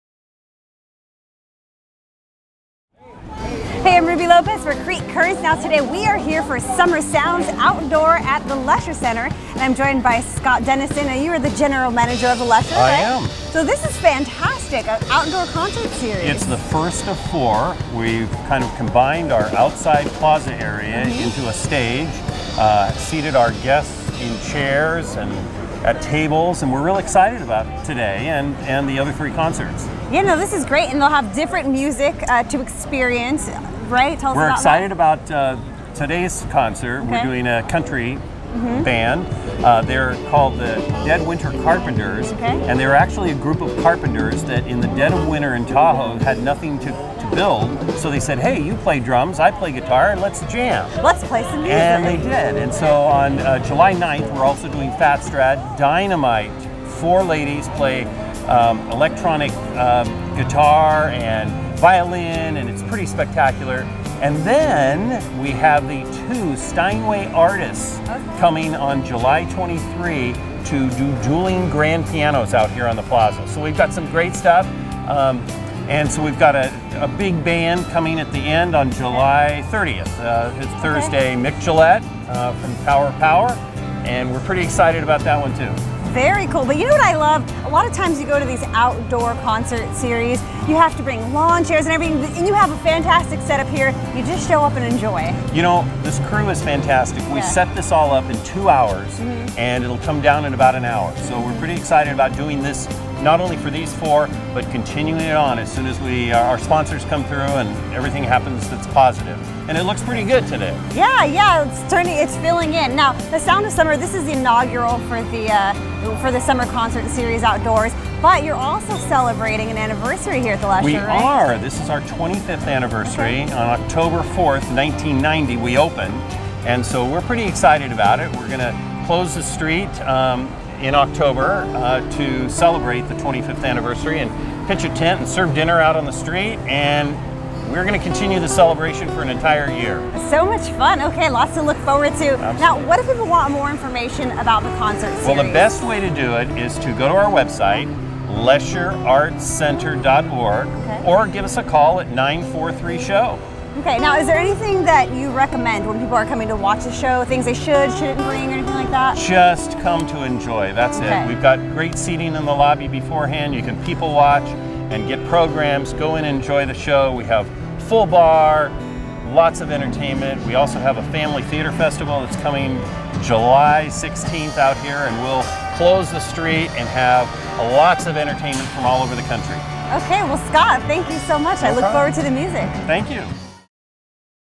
Summer Sounds Concert Series at the Lesher Center
Free music is coming to the Lesher Center this summer.  Learn all about the great line up when Ruby Lopez talks with Scott Denison, manager of the Lesher Center.